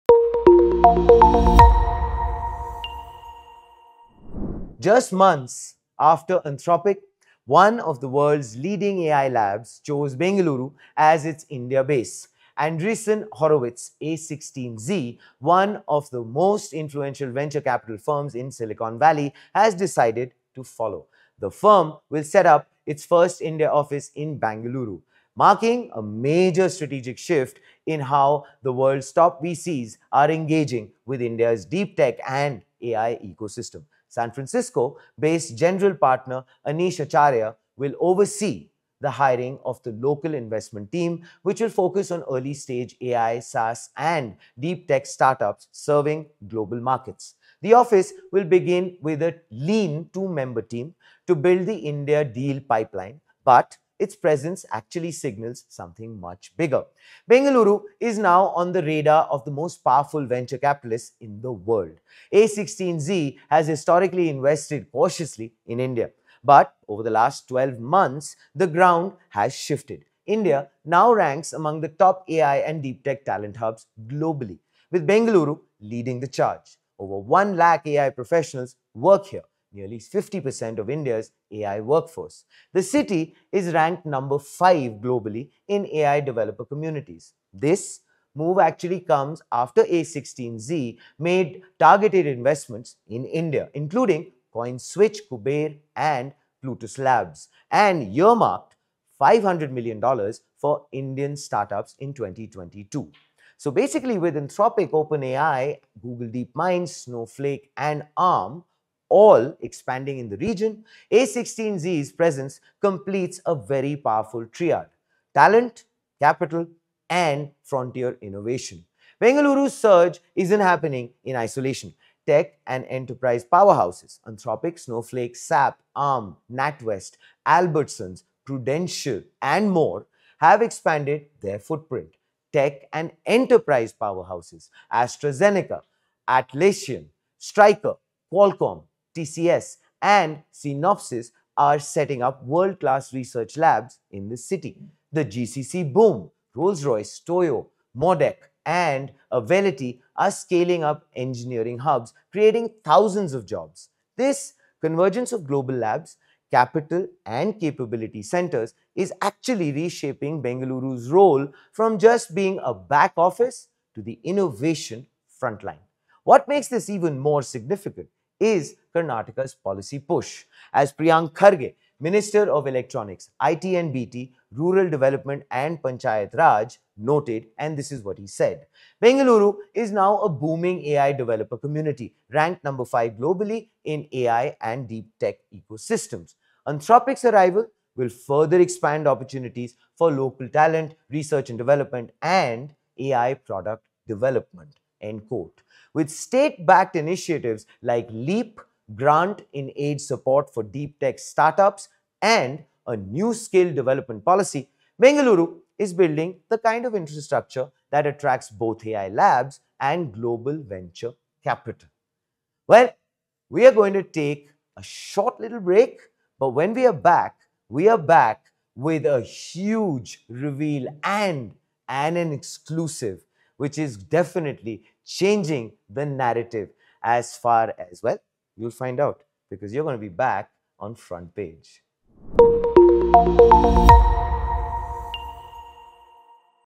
A16Z Enters India: Bengaluru Becomes Global Hub for AI, SaaS & Deeptech Startups
Bengaluru is becoming Silicon Valley’s next frontier. With Andreessen Horowitz (a16z) opening its first India office and Anthropic establishing a base here, India’s deeptech and AI ecosystem just went global. a16z, led by partner Anish Acharya, will focus on early-stage AI, SaaS, and deeptech startups from Bengaluru, now home to over 100,000 AI professionals and ranked #5 globally for AI talent. This shift marks India’s rise as a global innovation hub, fueled by top VCs, global tech giants, and proactive state policies. Watch how Bengaluru is transforming from a back office to a global AI and venture capital powerhouse.